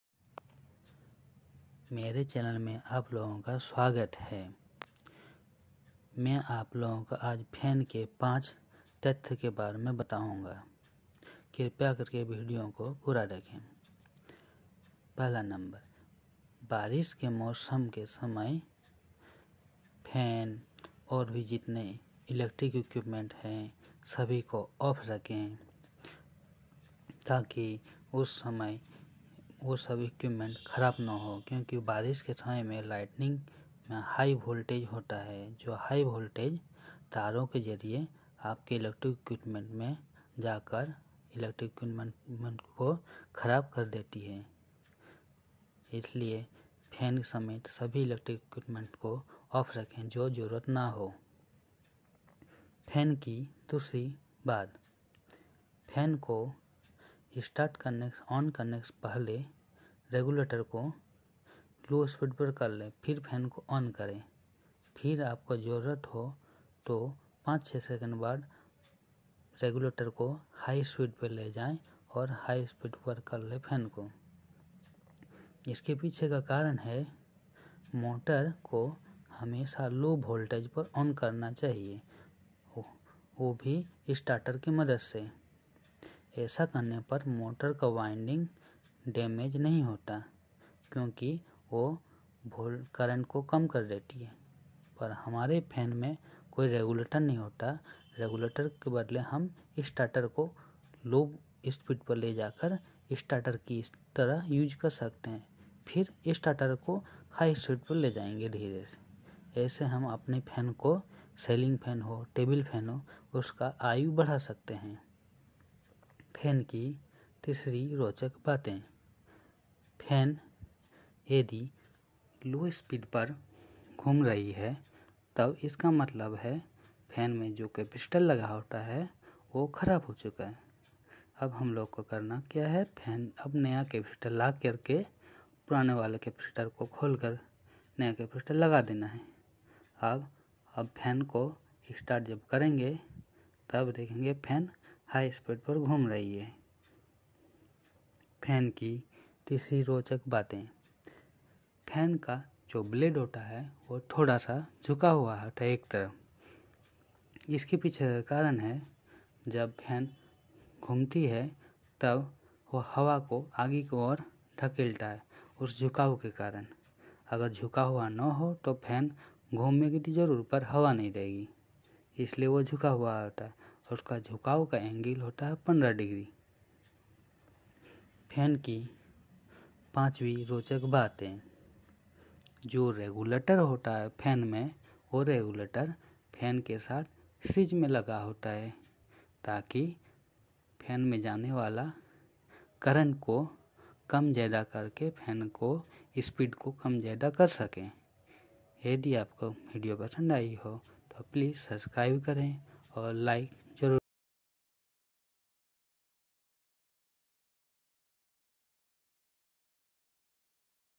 "ceiling fan slow speed problems what to do", "ceiling fan kam speed per ghumta Hai Kya kare?"
Fan ke bare me bataya hu: fan ko kese lightning se bachate. Fan ko kese kharab hone re bachaye.
Fan slow speed per keyo ghumta ha kore kese thik kare.
Fan ka blade keyo therha ( band) hota ha.
# lightning
# fan life increase
# fan slow speed problems
# fan blade band
# Running on induction process.
# Fan is actually 2 phase motor not single phase. Inside motor make 2 phase but out side of fan only single phase obrain.